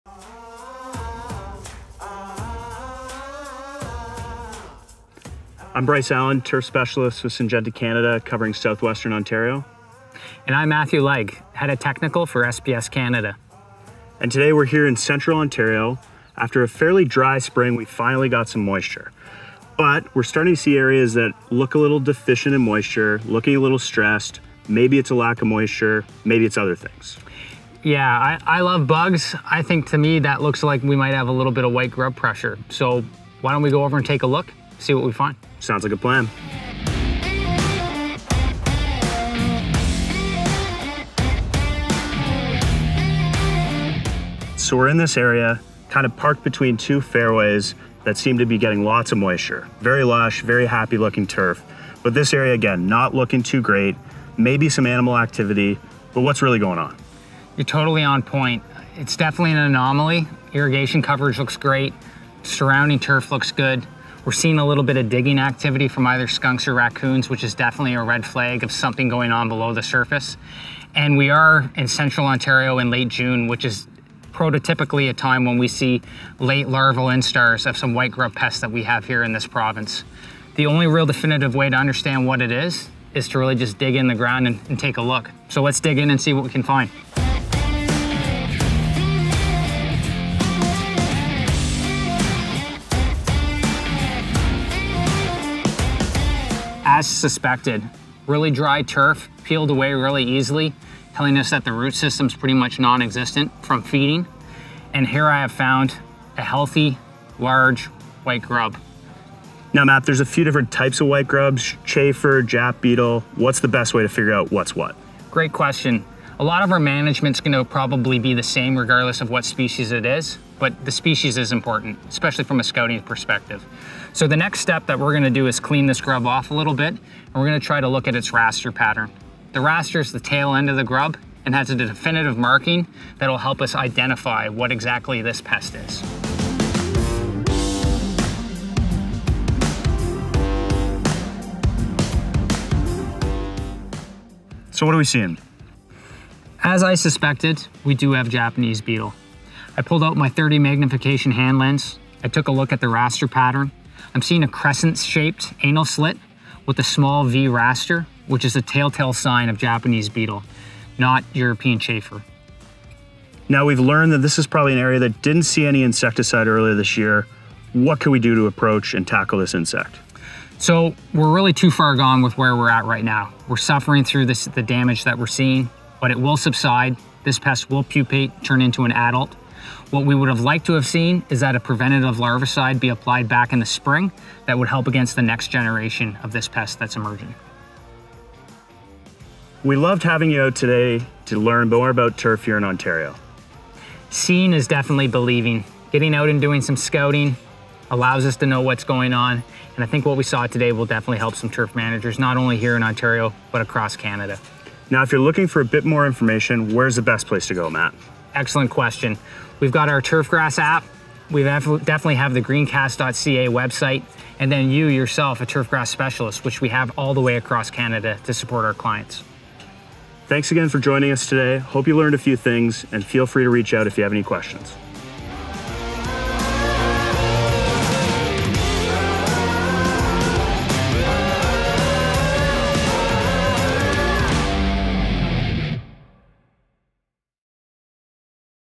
Digging Deeper: Looking for grubs | Turf TV
In this episode of Turf TV, SPS Technical Lead, Matt Legg and Turf Specialist, Bryce Allan investigate the cause of stressed-out turf at a central Ontario golf course. Get their take on how to scout for grubs and prevent pest pressure in the future.